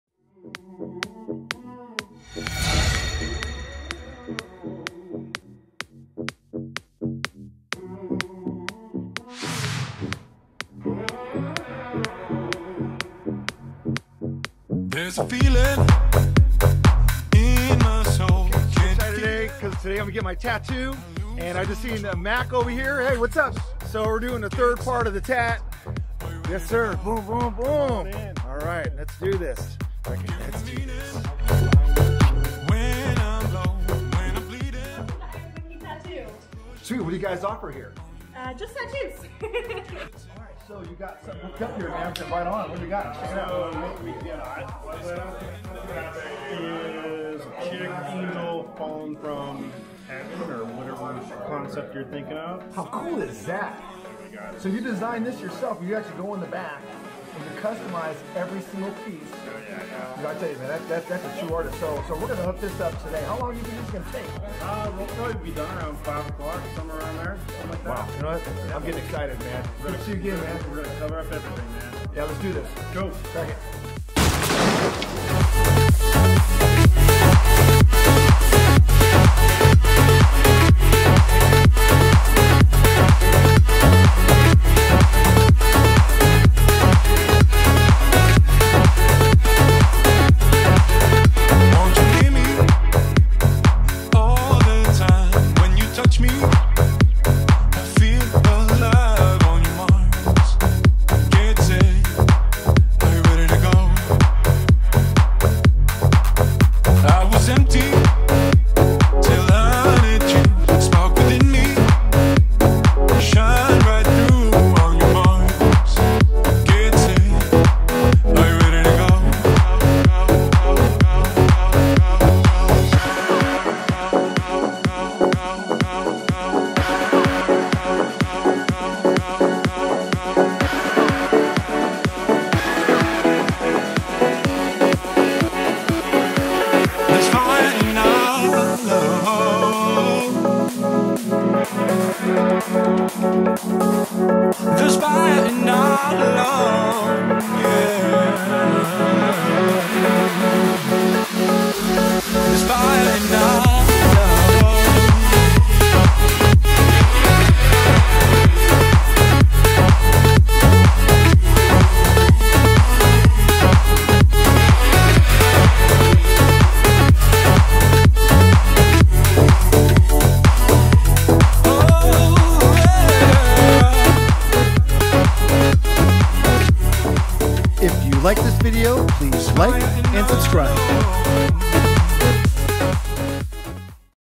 Tattoo at the IRON MONKEY
Today is a great day because I’m with my buddy Mac at the iron monkey tattoo. I’m getting the third part finished on my leg.